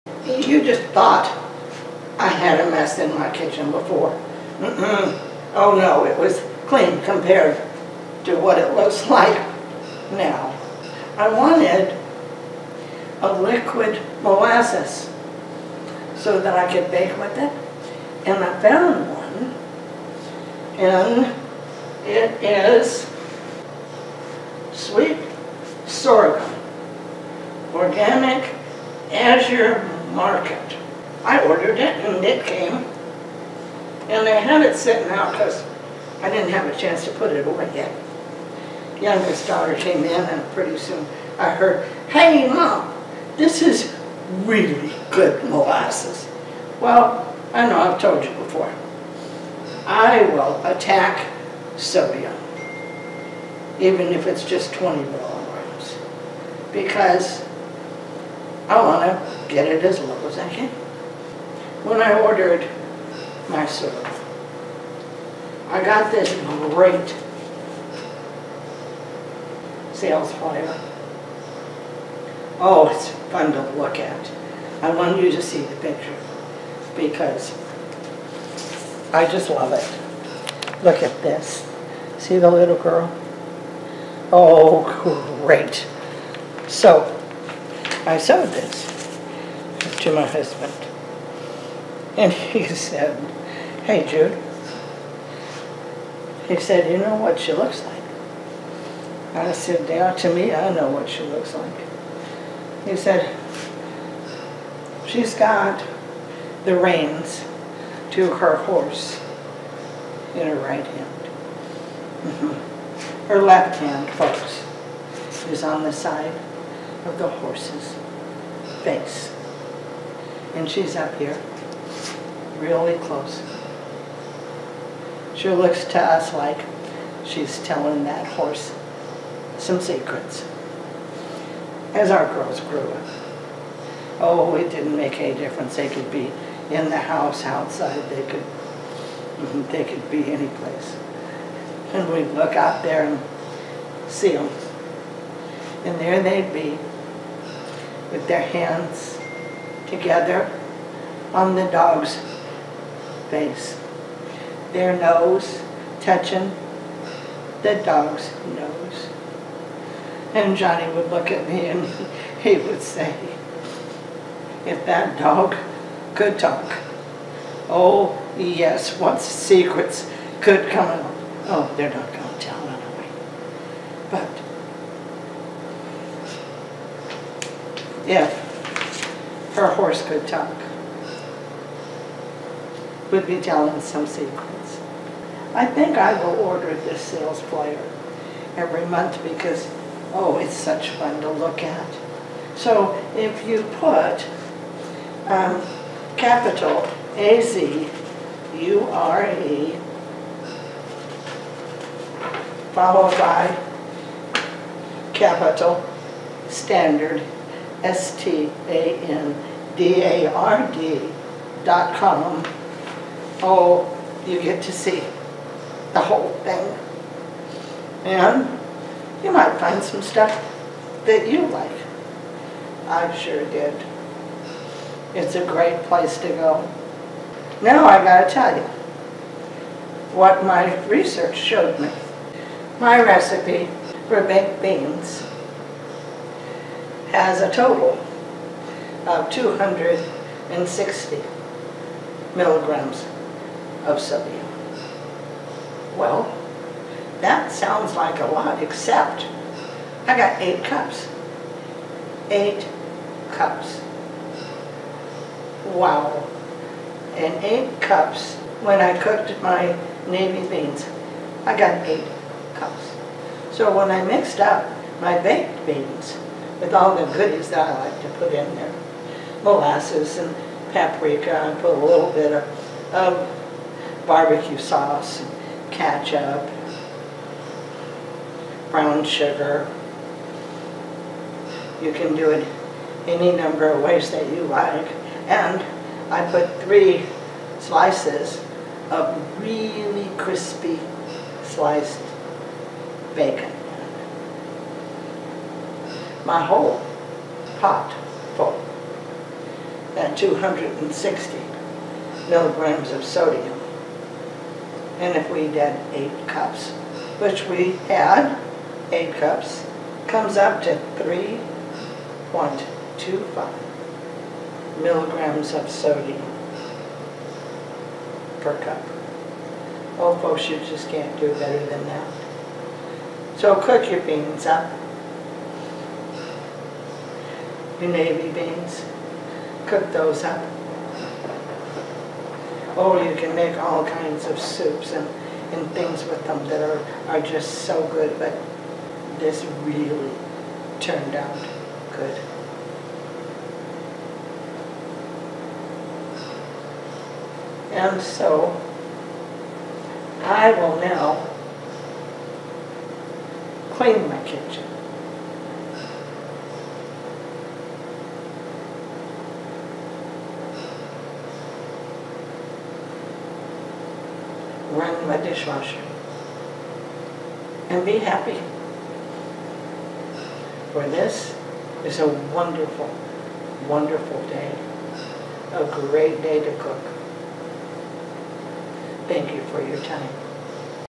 Very tasty Baked Beans
Baked beans - less than 4 mg sodium per cup. Bush's baked beans 440 to 640 mg per cup. Grandson said "Grandma, these baked beans are really good."

Link to all my LOW SODIUM COOKING COURSES.